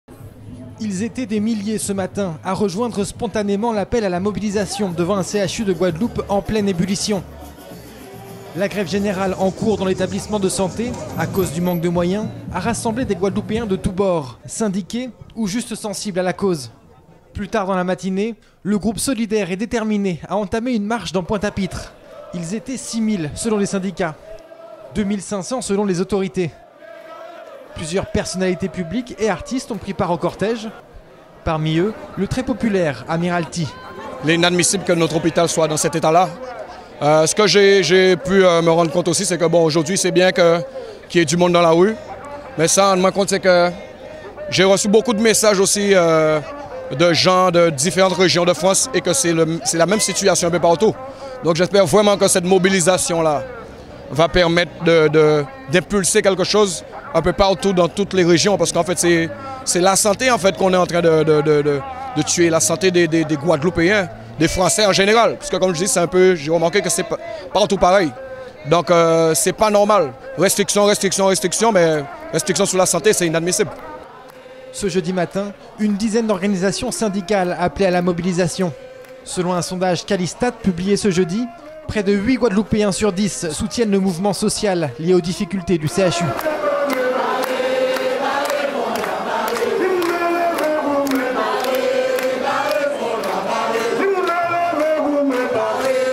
🔴 Une mobilisation historique aujourd'hui pour le CHU de Guadeloupe
"Les restrictions sur la santé, c'est inadmissible" déclare Amiral T

Une foule solidaire a inondé les rues de Pointe-à-Pitre ce matin ⤵️